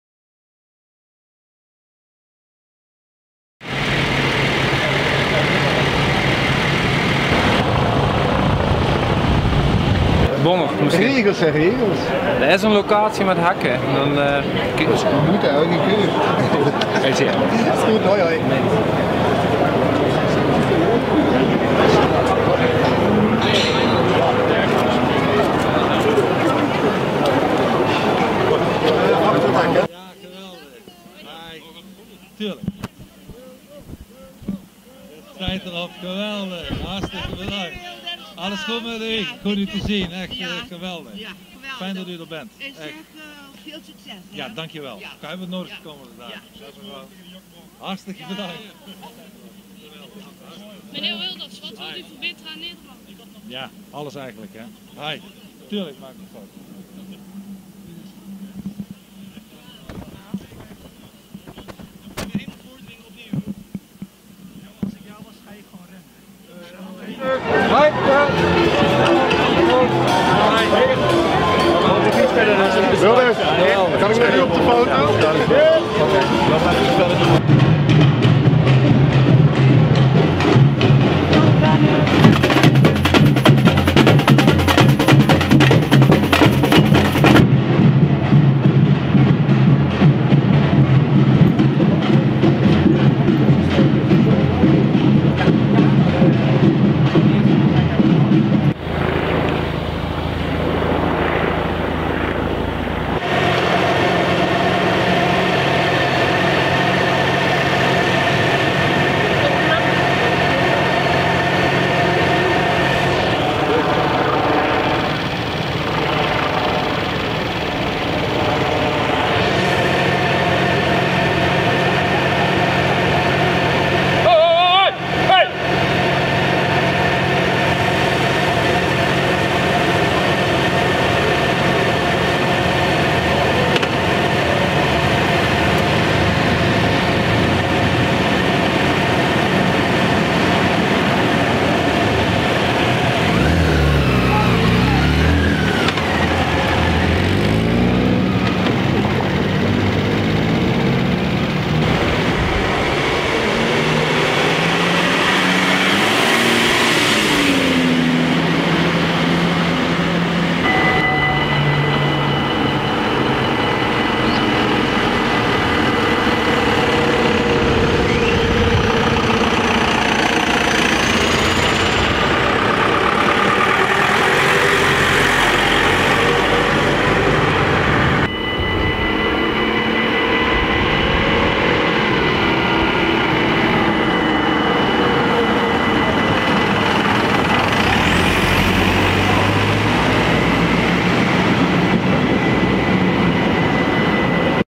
[Heerlen] Geert Wilders komt flyeren: grote politieinzet, begeleiding en interview!
Deze video is gesponsord door Multicopy Maastricht